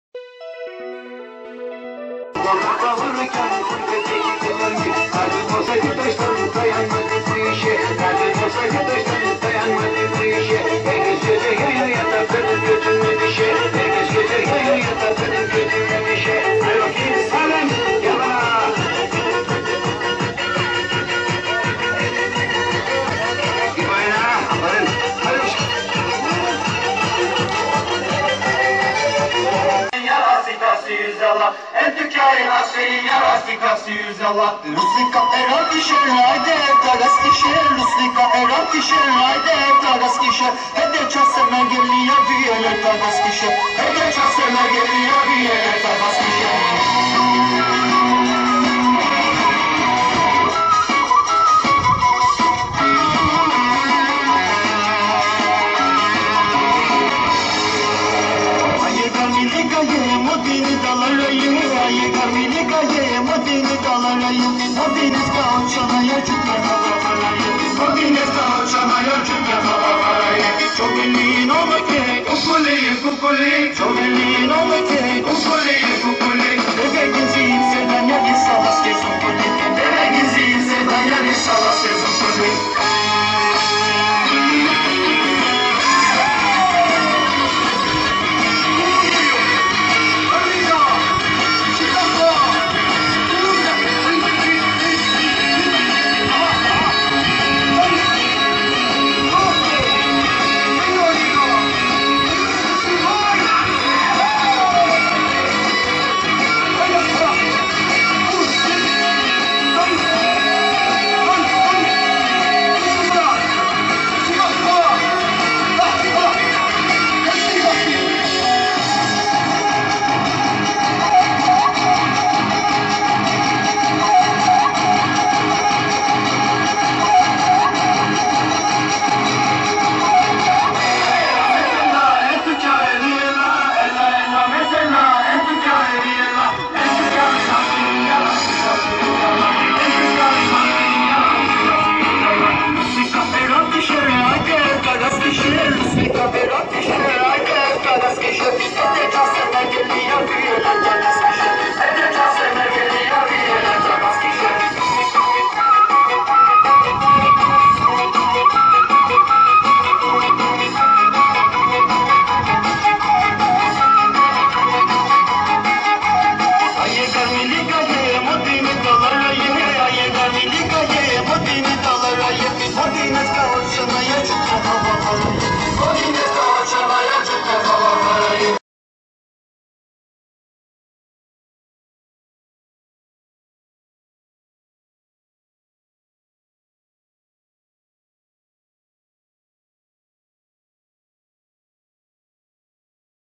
KARASU KIZILCIK İLK ve ORTAOKULU ETKİNLİK KH
Haziran ayının ilk haftası olarak kabul görülen Hayat Boyu Öğrenme Haftası etkinlikleri kapsamında Karasu Kızılcık İlk ve Ortaokulu’nda “7’den 70’de horon” etkinliği gerçekleştirildi. Velilerin ve mahalle halkının da öğrencilere eşlik ettiği etkinlikle doyasıya eğlenildi. - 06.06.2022 - HABER